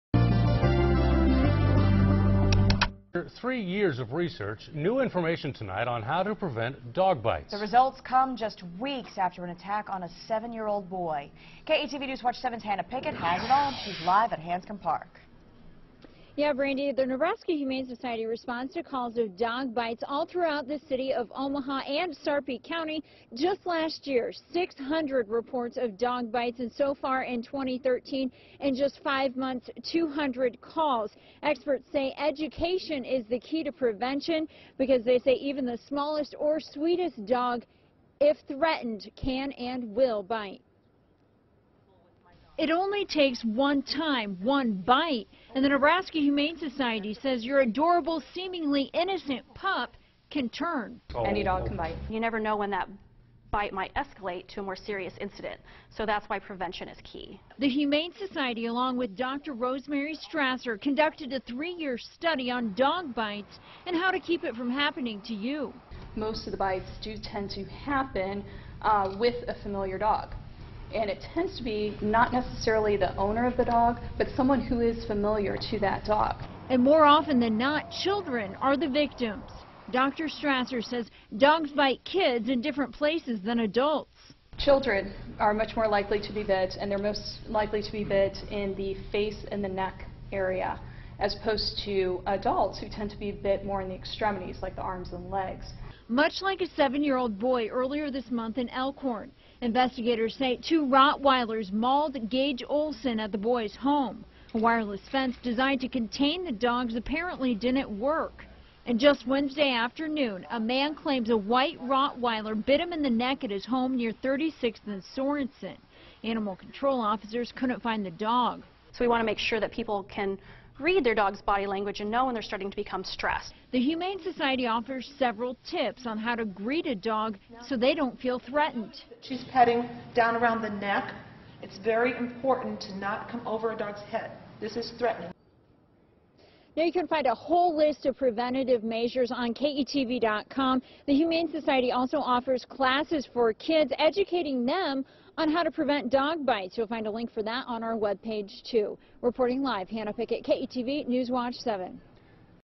Animal experts educate community on dog bite prevention
Animal control officers respond to hundreds of dog bite calls each year, but experts say dog bites can be prevented with animal education.